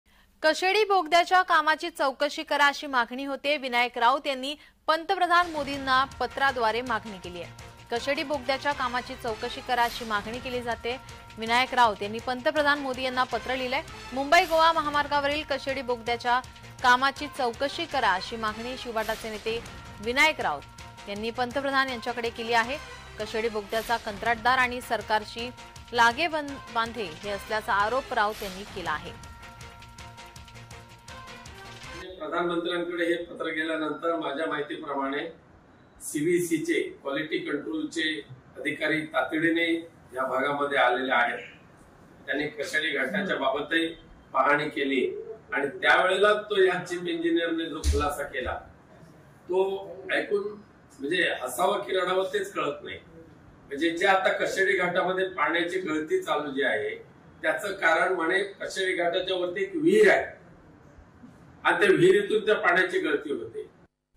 Vinayak Raut To PM Modi | 'कशेडी बोगद्याच्या कामाची चौकशी करा' | Marathi News
'कशेडी बोगद्याच्या कामाची चौकशी करा'
विनायक राऊतांचं पंतप्रधान मोदींना पत्र